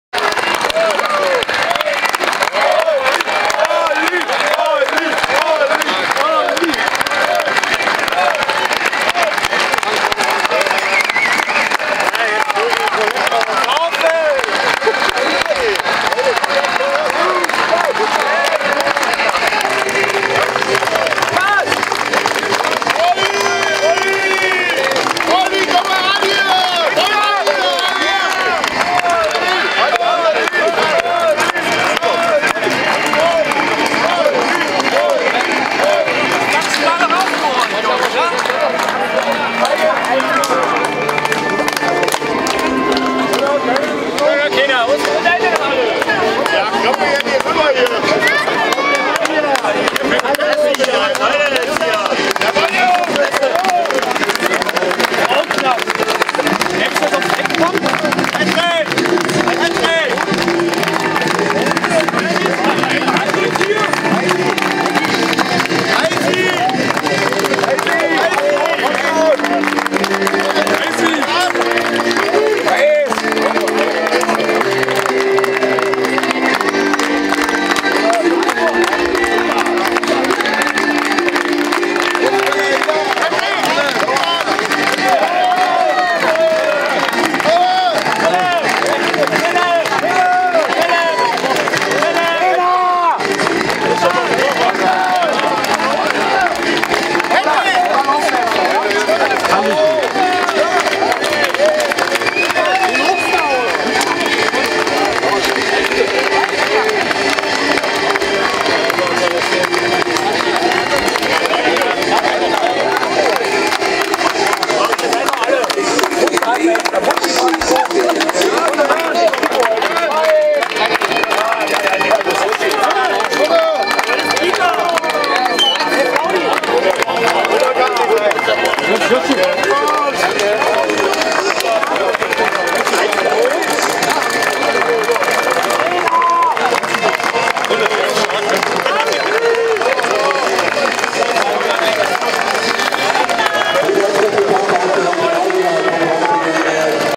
30.Spieltag nach dem Spiel in Bautzen die Manschaft Feiert mit den Fans BFC Dynamo
FSV Budissa Bautzen - BFC Dynamo